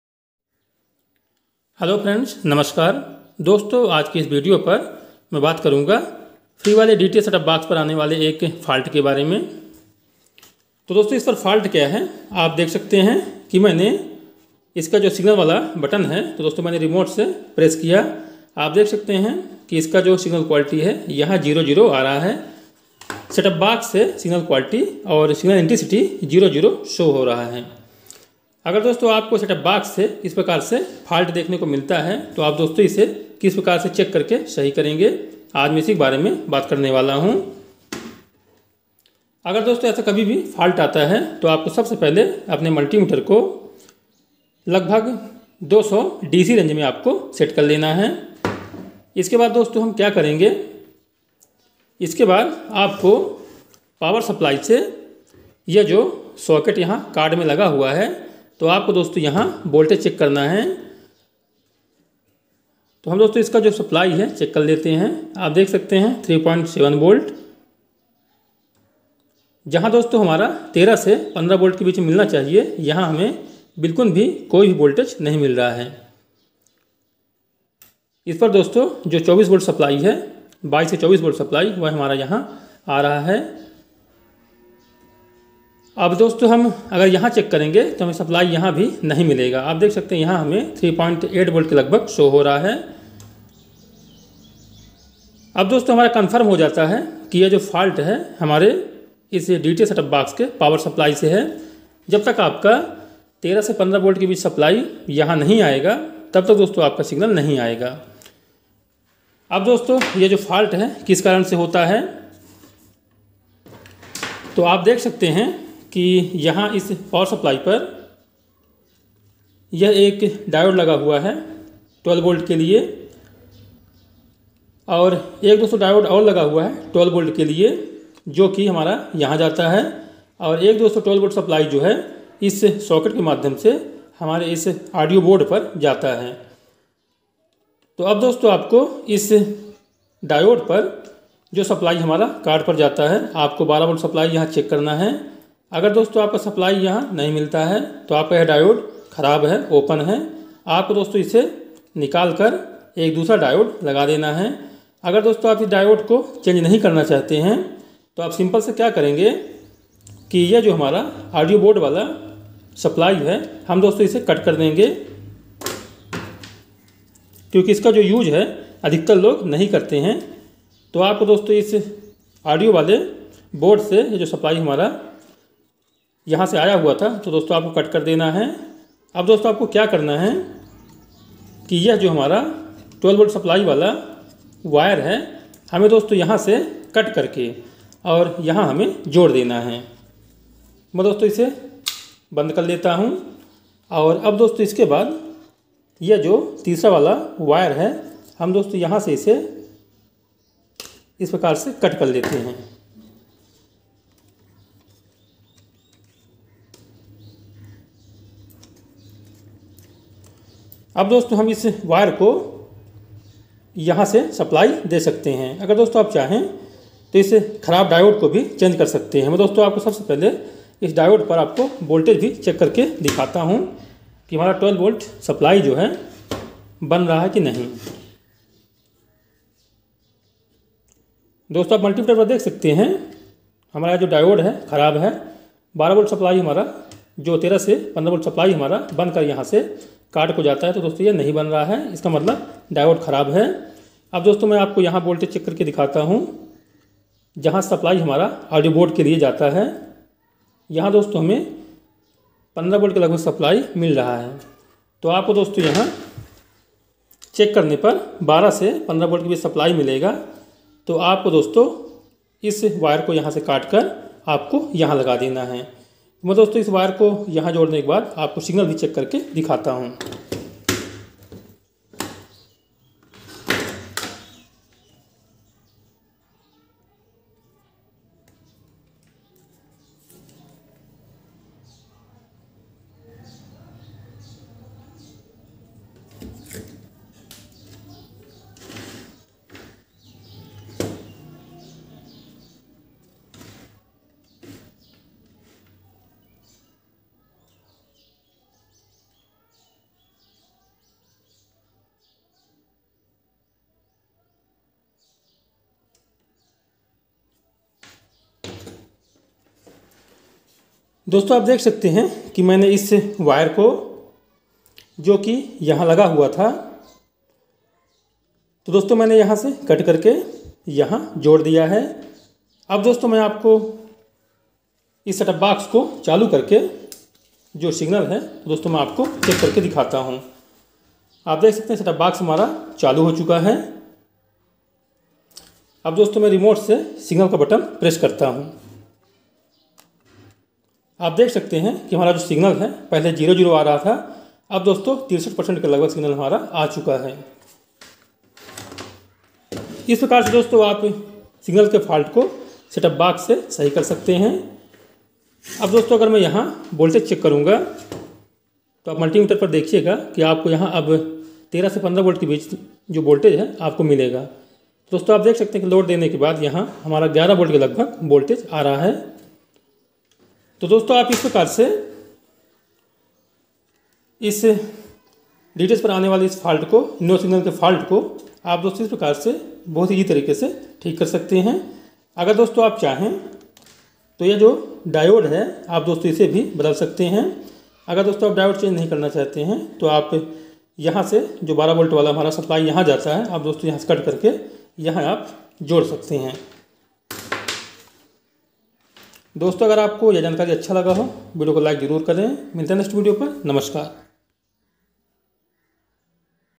free dish setupbox पर Signal Quality zero की समस्या को ठीक करना सीखें।
free dth
free dish setupbox signal falt
free dish setupbox repairing
dth setupbox signal problem